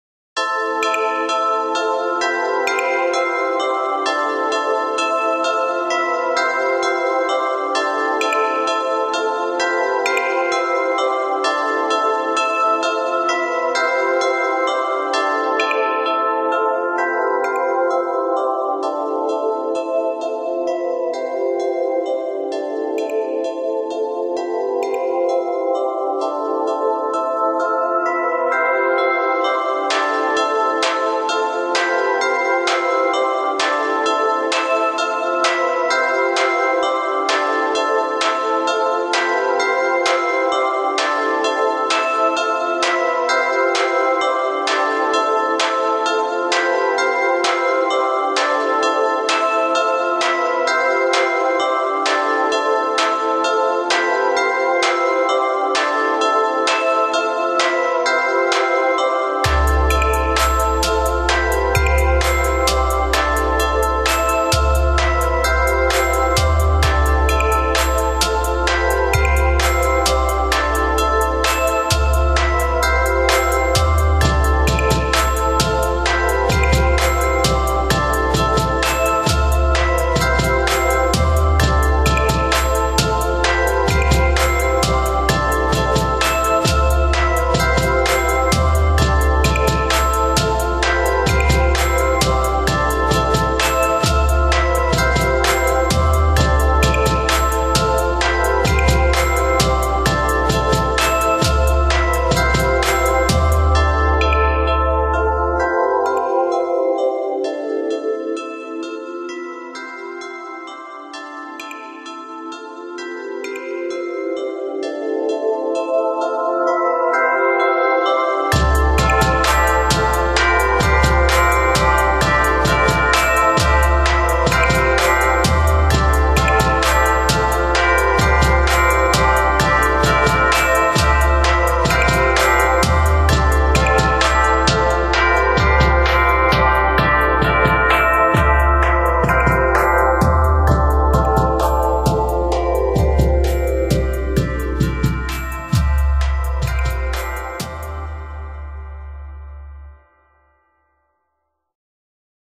Piano x Drum Beat
Just something I created using a piano and a drum pattern.
Criticism welcome!